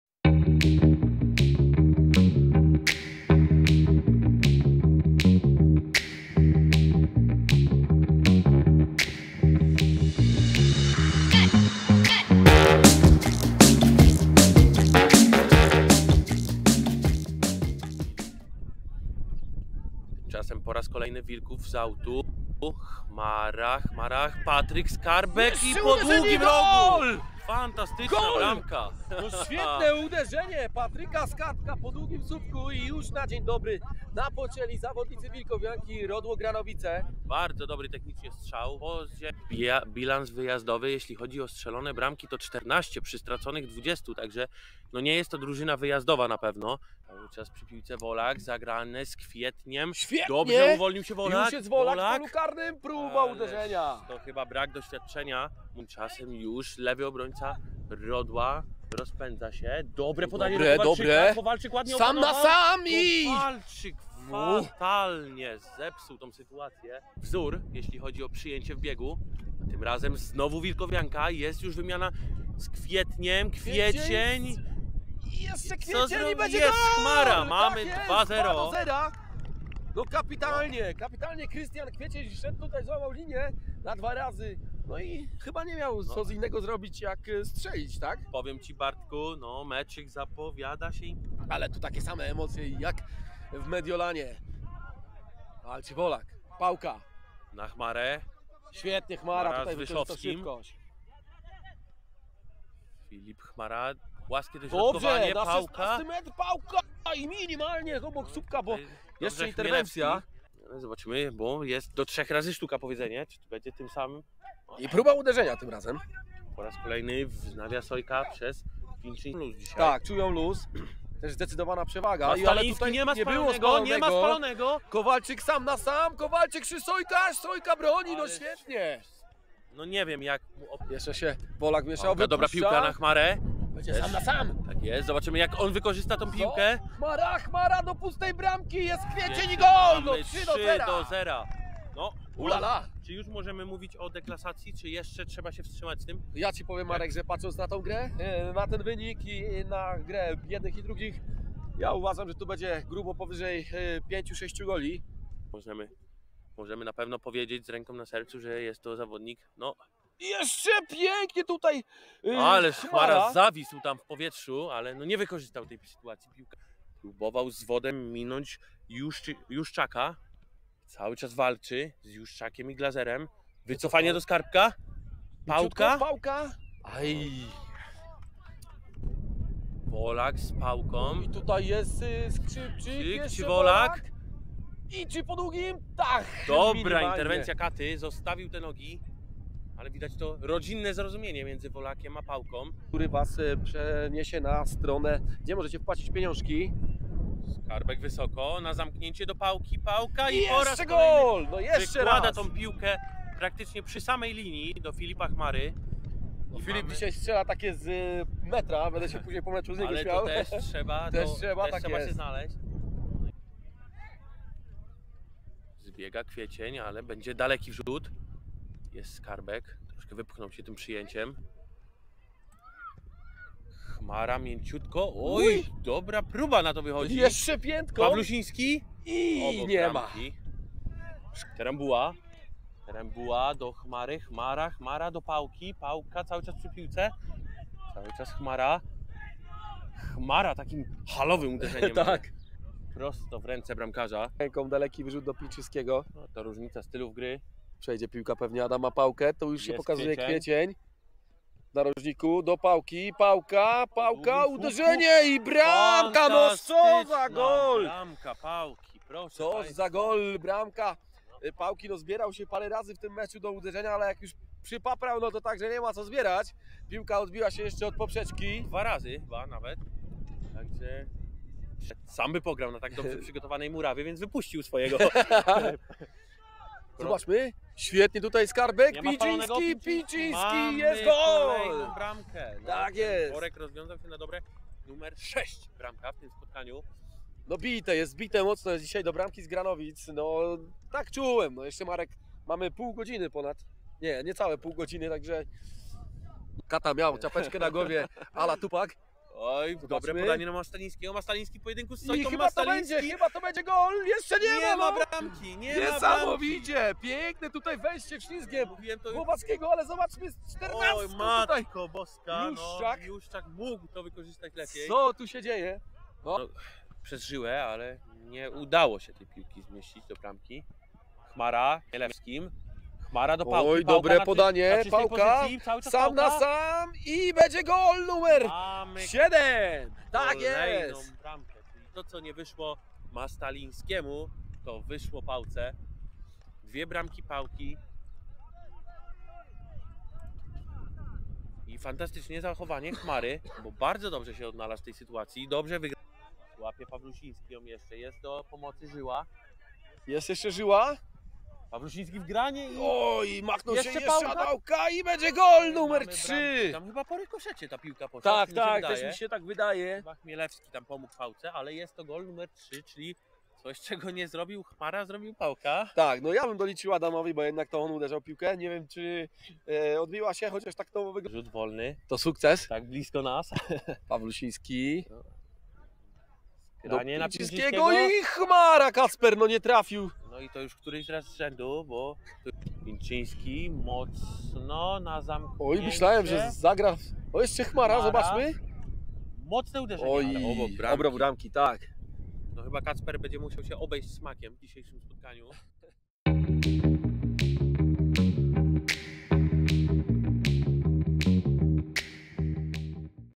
[A-KLASA LEGNICA GRUPA III] WILKOWIANKA WILKÓW - RODŁO GRANOWICE 8:0 (SKRÓT SPOTKANIA) - 21 KOLEJKA
Wilkowianka nie zwalnia tempa i zbiera kolejny skalp!

Po wysokiej wygranej w Lipie gościła na swoim terenie Rodło Granowice i wykonała zadanie bez zarzutu. Gospodarze rozpoczęli strzelanie błyskawicznie, by po kwadransie prowadzić dwoma golami.

Ostatecznie licznik zatrzymał się na ośmiu, a najlepiej mecz z Rodłem będzie wspominał Adam Pałka, który popisał się hat - trickiem. Trzy zdobyte punkty przybliżyły Wilkowiankę do podium, o które powalczy już w najbliższą niedzielę w Zagrodnie.